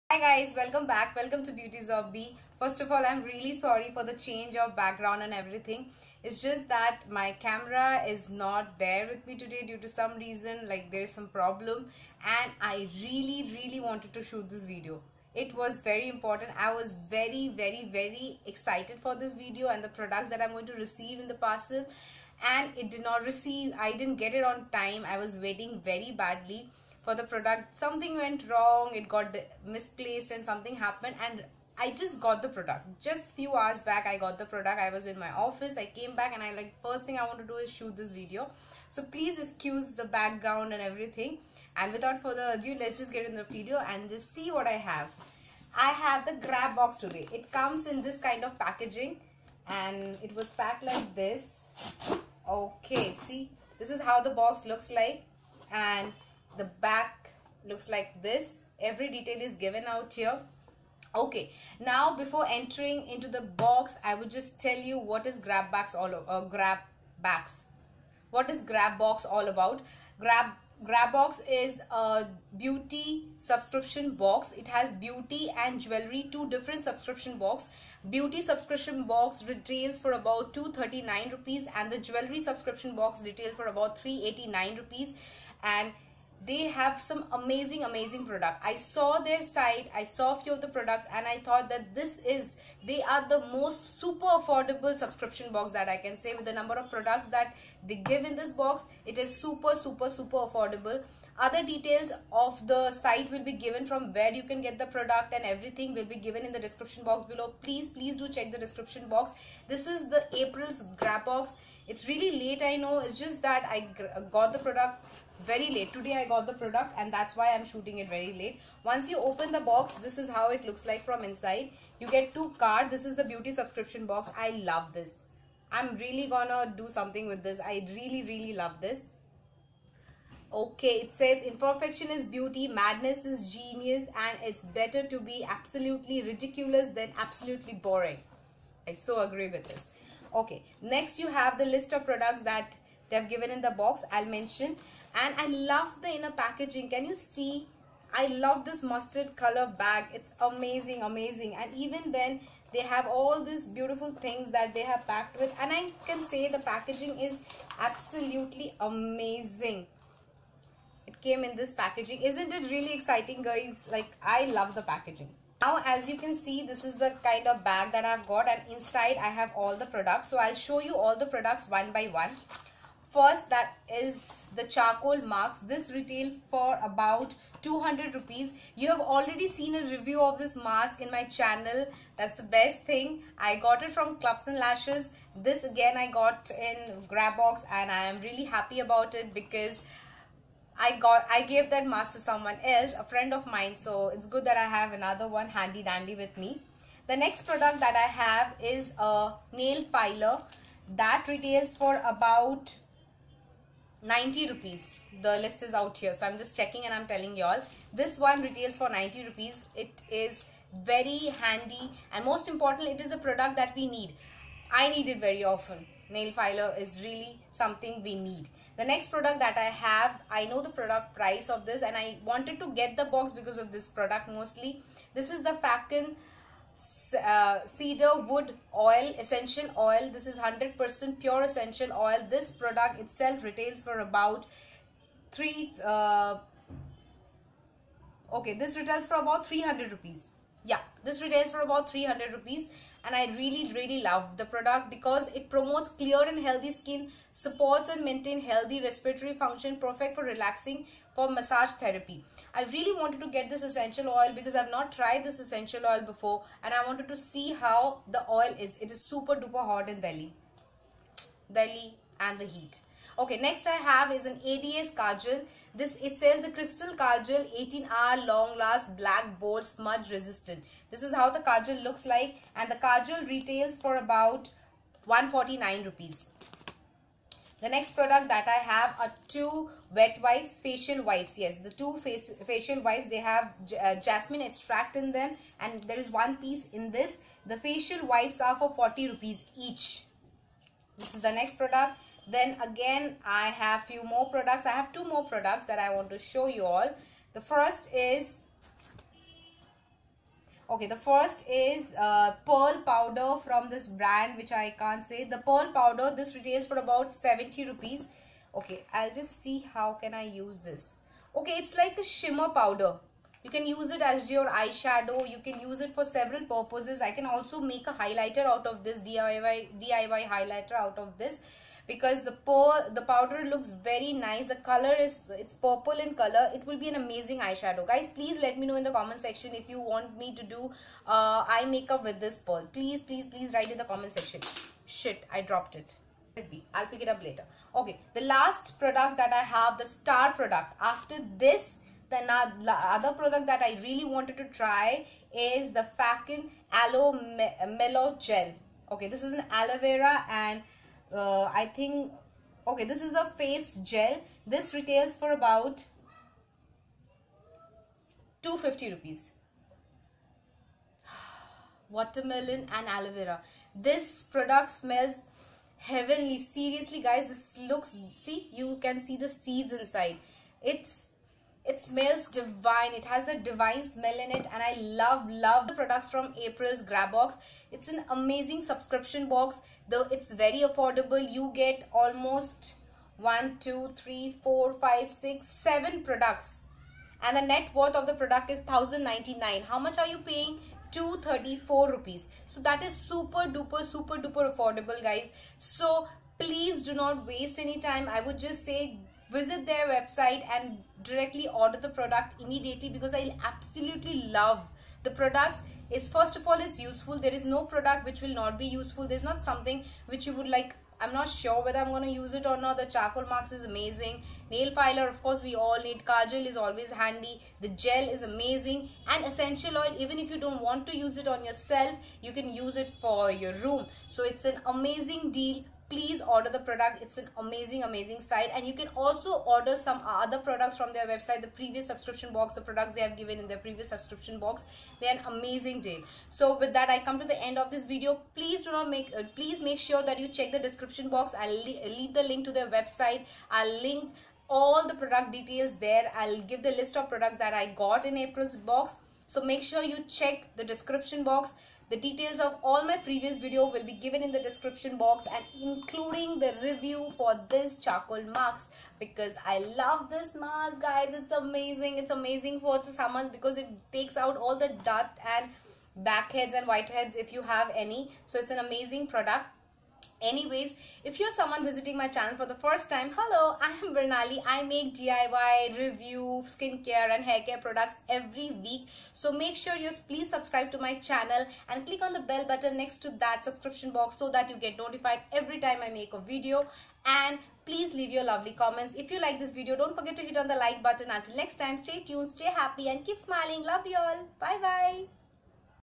#81 Beauty Grabox April 2018 | Most Affordable| At 239 only | Unboxing & Review
Grabox is Faconn's monthly subscription box and have two varients i.e. Beauty Grabox and Jewel Grabox priced at Rs.239/- and Rs.389/- respectively plus Rs.60/- shipping. They have unique subscription box at most affordable prices.

Price : Rs. 239 + 60 for shipping

#18 FACE PACK TO PREVENT PIMPLES, RASHES, AND ACNE

XOXO
FROM B